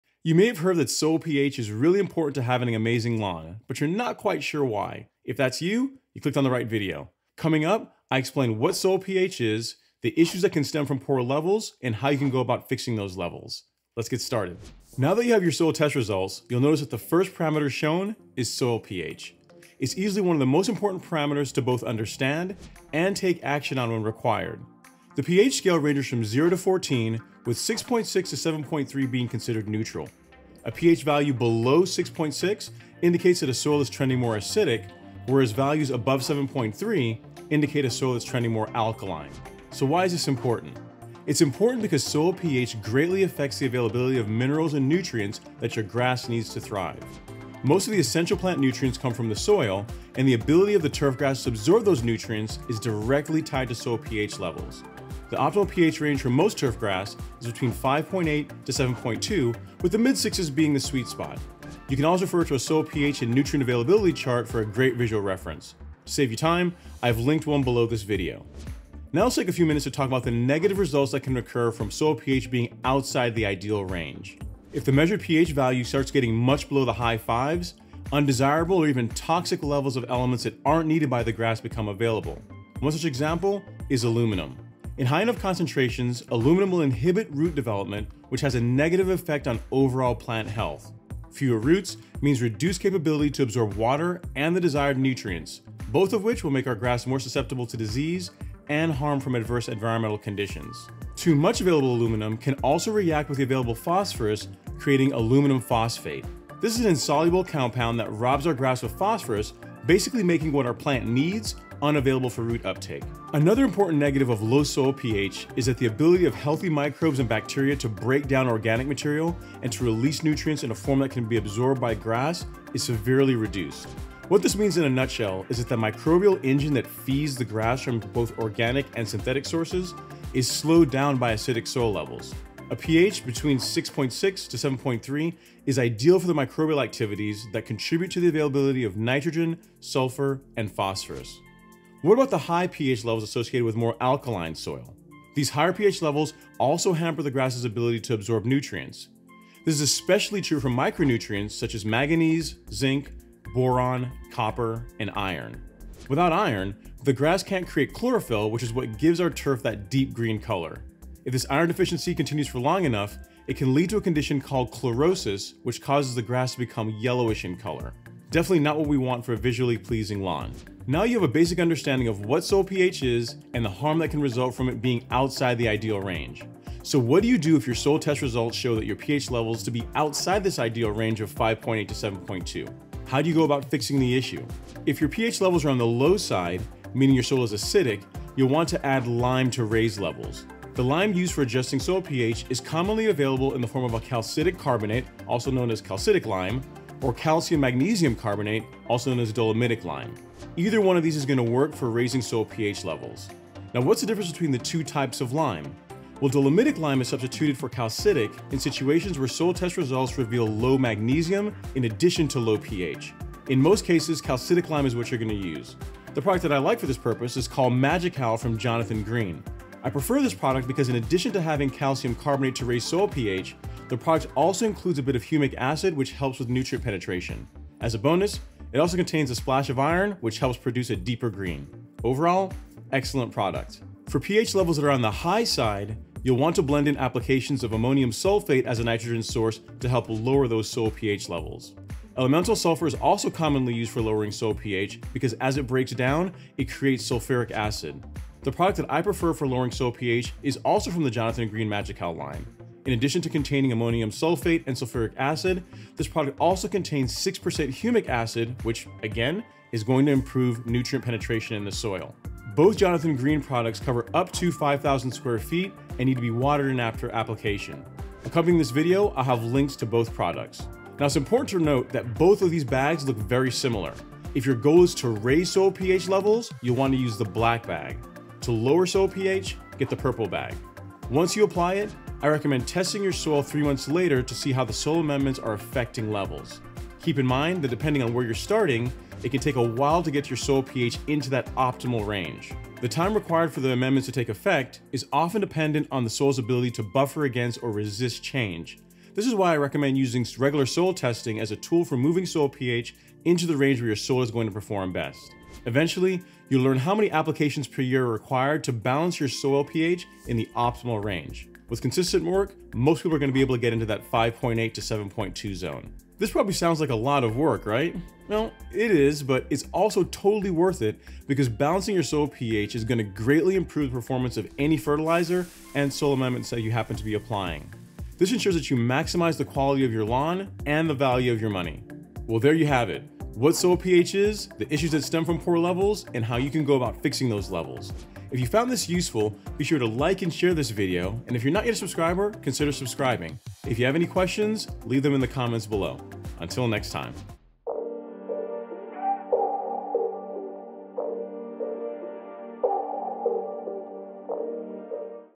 Soil pH Explained - [The Faster Way to a Greener Lawn]
Soil pH is one of the most important parts of soil test results. It's an indication of the acidity or alkalinity of soil and is measured in pH units ranging from 0 - 14. It has a huge impact on nutrient availability which effects how your lawn is able to make use of those nutrients. Keeping soil pH in the optimal range grass is the faster way to greener lawn. When it's correct, your lawn will make better and more efficient use of fertilizer and other soil amendments.

In this video I cover, what soil pH is, the lawn care issues that result from it being outside of the ideal range, and how to amend the soil to move pH into the ideal range.

*Apologies for the misspelling of "alkaline" at the 38 second mark.*

✅ Everything you need for perfect lawn at the Golf Course Lawn Store

Video Contents ---
0:00 Introduction
0:16 What is Soil pH
0:26 pH Scale
0:40 Why pH is Important
1:10 Issues from Poor pH Levels
3:01 How to Fix Acidic Soil
3:55 How to Fix Alkaline Soil
4:22 Product Coverage
4:40 Important Considerations
4:55 Correcting Over Time
5:15 The Payoff

Vlogging Lens